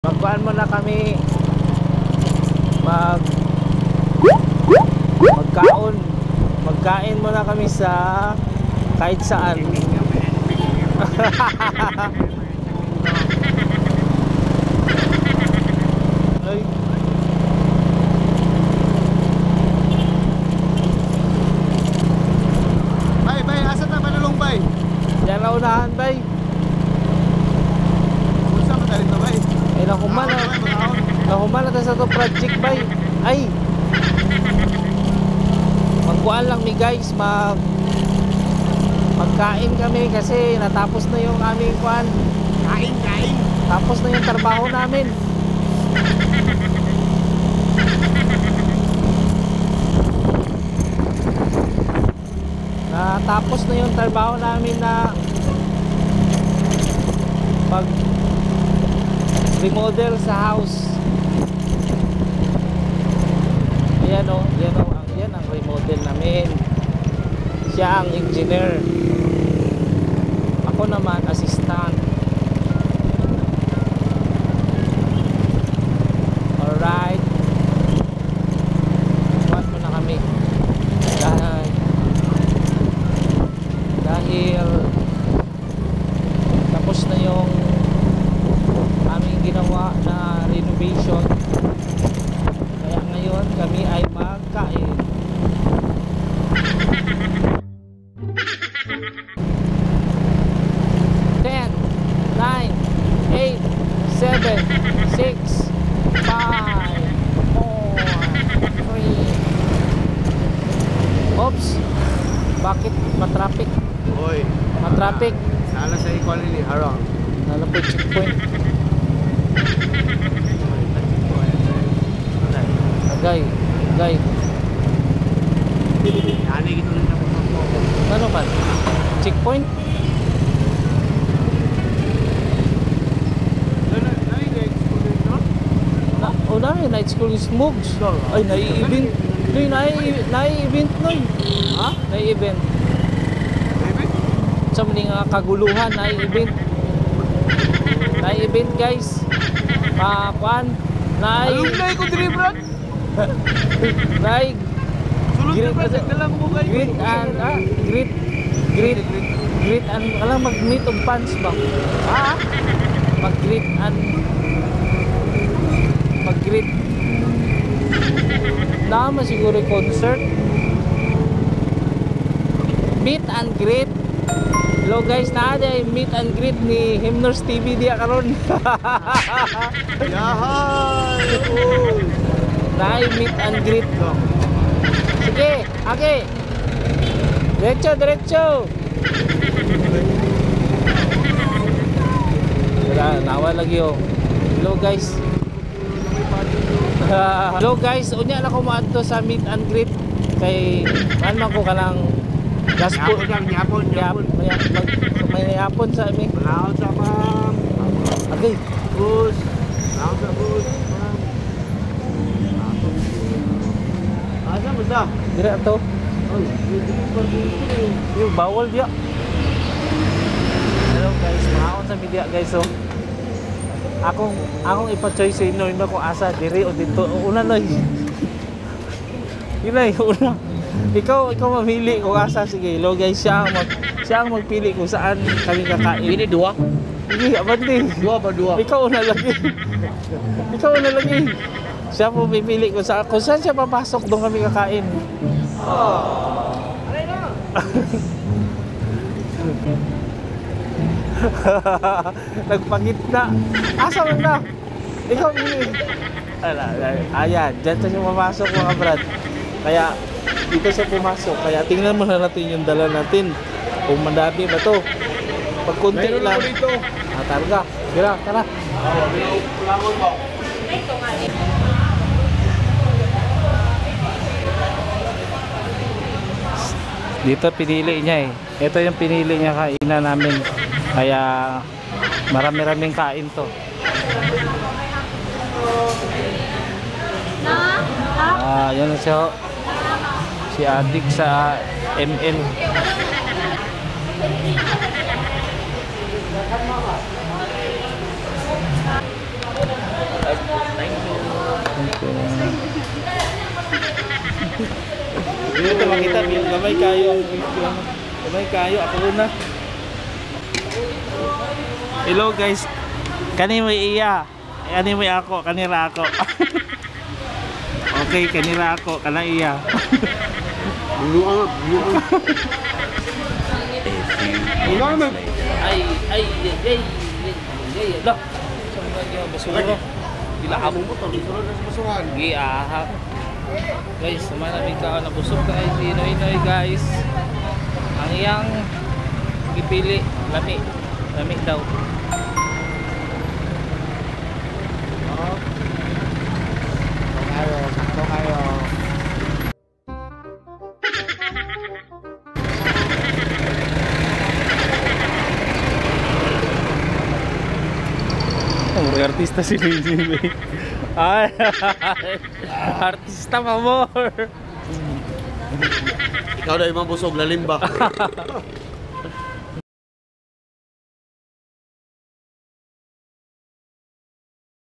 MEET AND GREET | KUMAIN AT NABUSOG
Pagkatapos ng renovation sa bahay, kain nanaman kami. Kasama ko ang si Nonoy "The Engineer"

Salamat po. Peace yo!